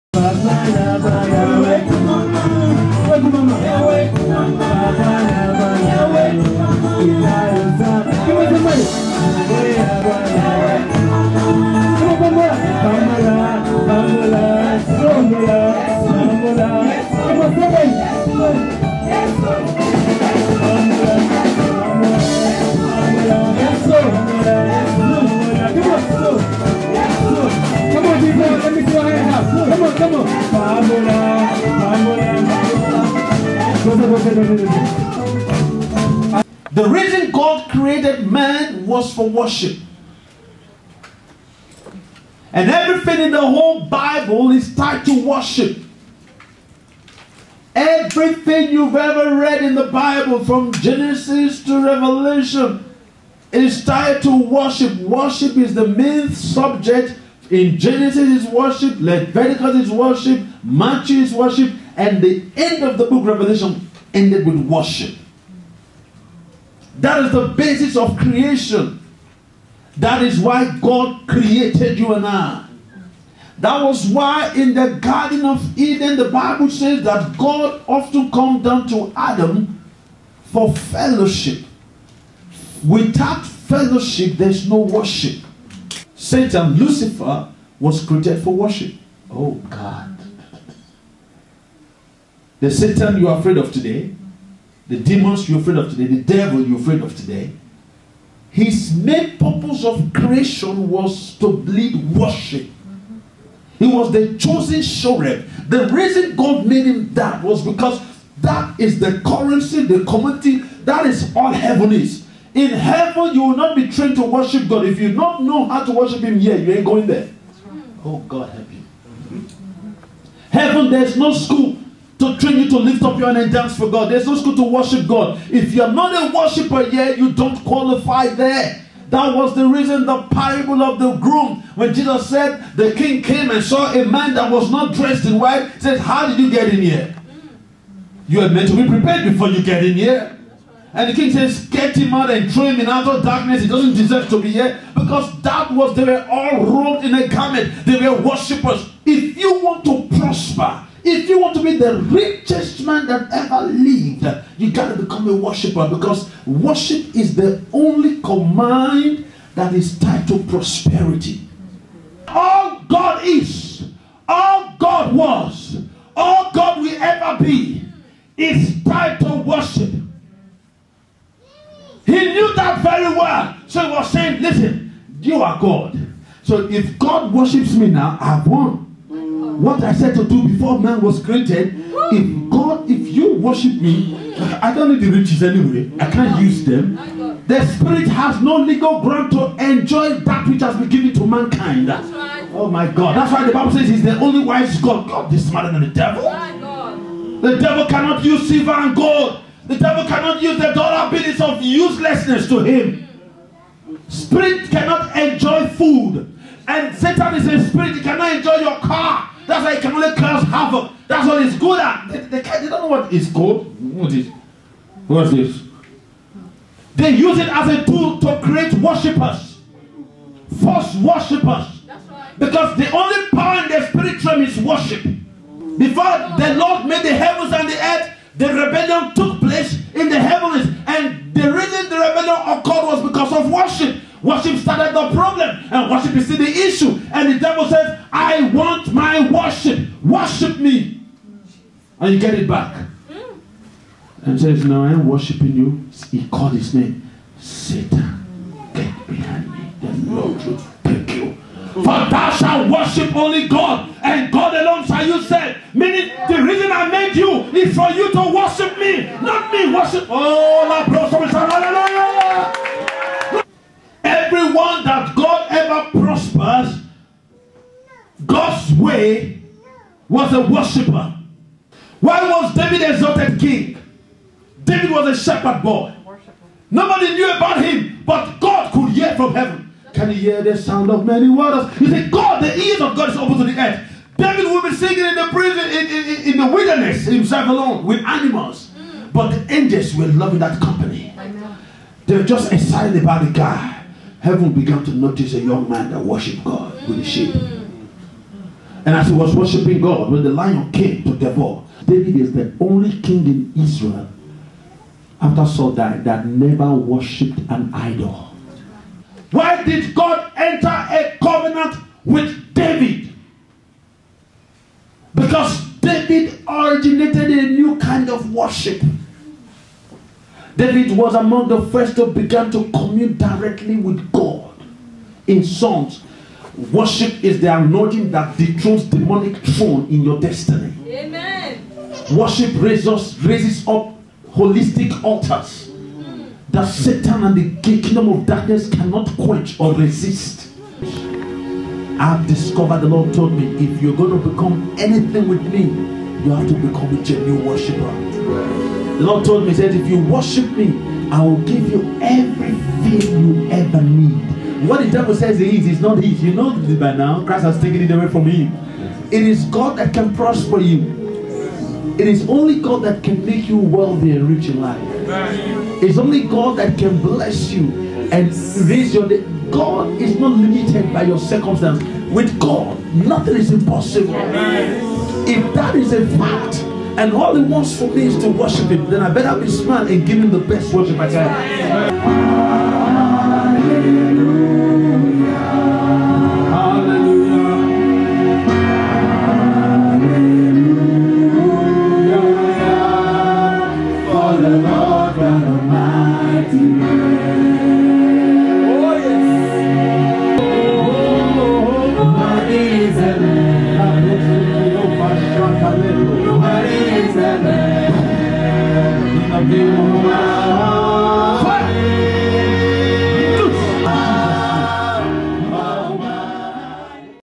Prophet William: "Worship"
A sermon on worship at the church, "Fountain Ramah".